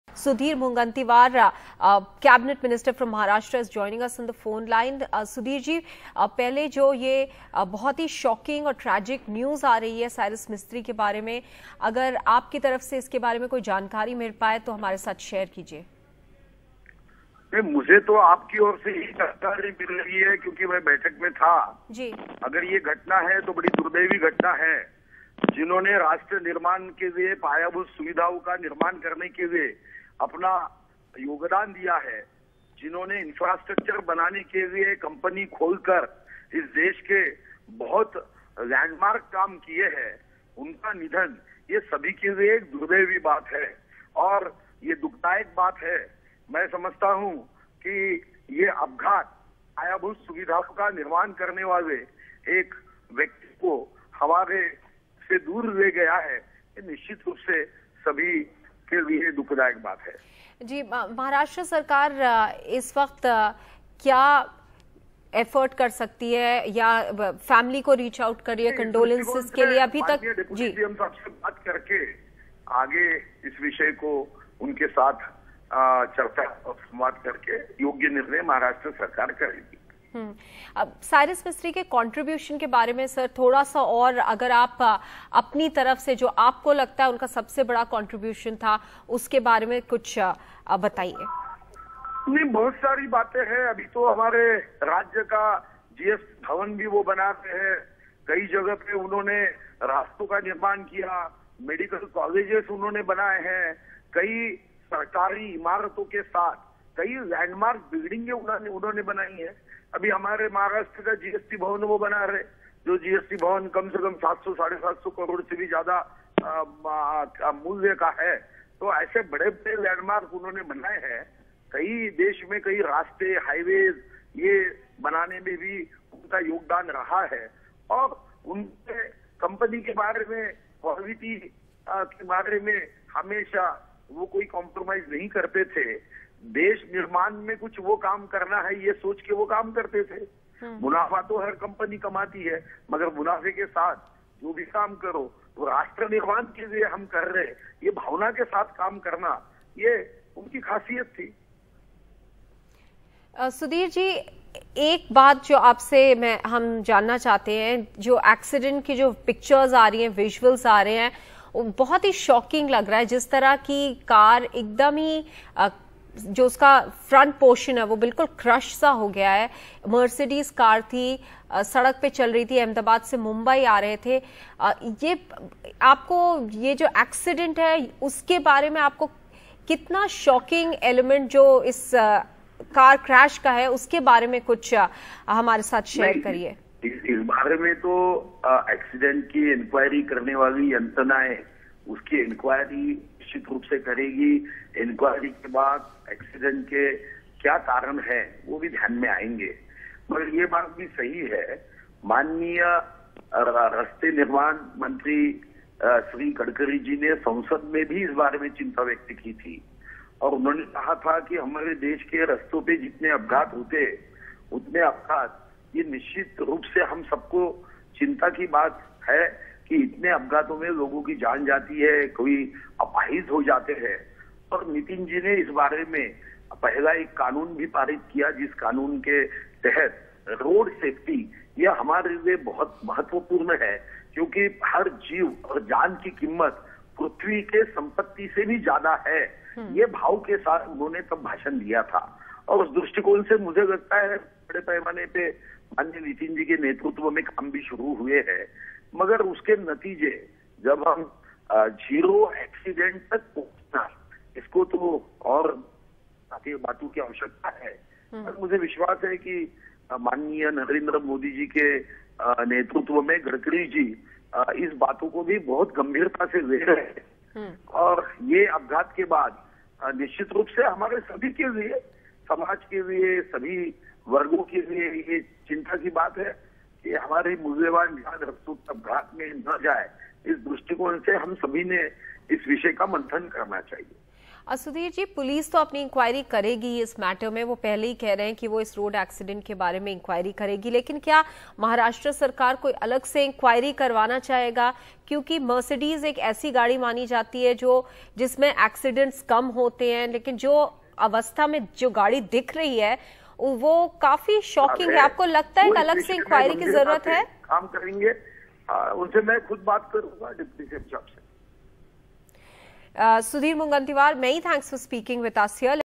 "Worked With Spirit Of Nation Building": Maharashtra Minister On Cyrus Mistry's Death
Cyrus Mistry Dies: "He worked with a spirit of nation-building," says Maharashtra Minister Sudhir Mungantiwar on former Tata Sons Chairman Cyrus Mistry's death in a car accident near Mumbai.